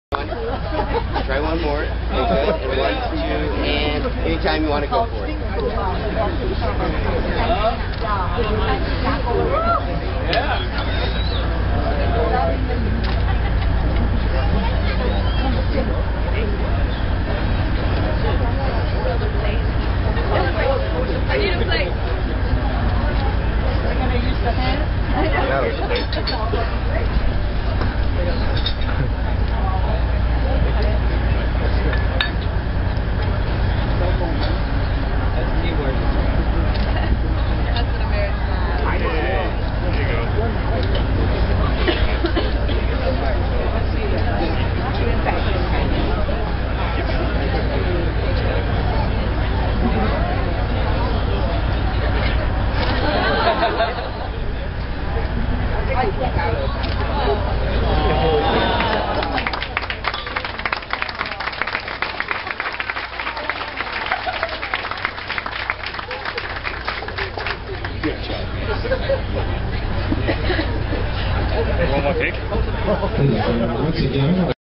Jack and Natalie cut the cake
jackalie cut their wedding cake.